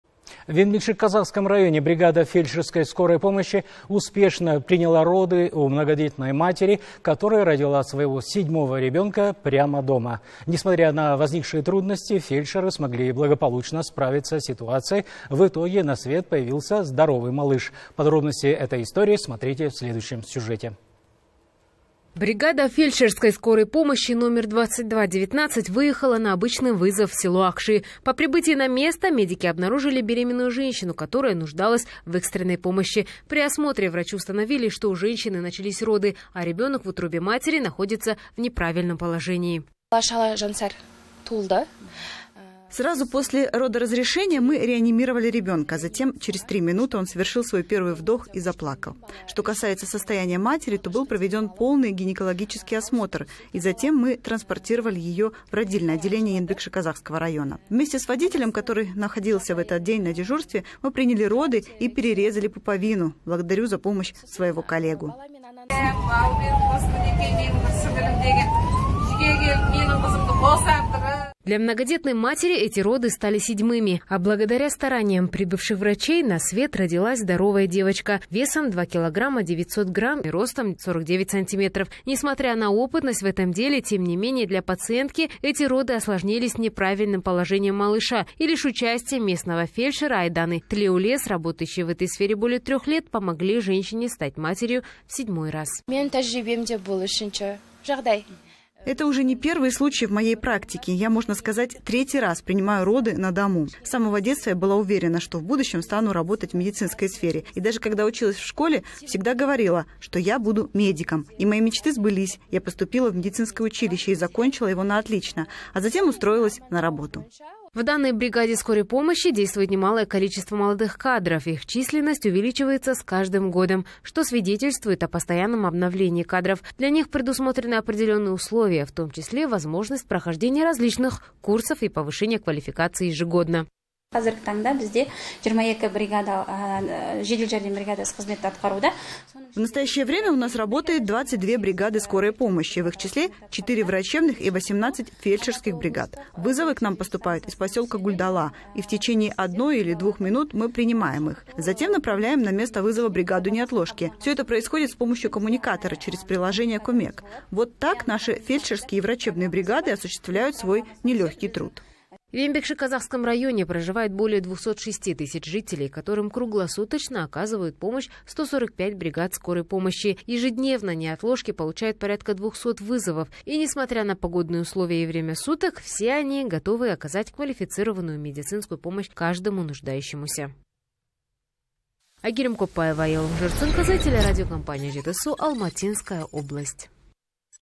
В Енбекшиказахском районе бригада фельдшерской скорой помощи успешно приняла роды
В Енбекшиказахском районе бригада фельдшерской скорой помощи успешно приняла роды у многодетной матери, которая родила своего седьмого ребенка прямо дома. Несмотря на возникшие трудности, фельдшеры смогли благополучно справиться с ситуацией, в итоге на свет появился здоровый малыш.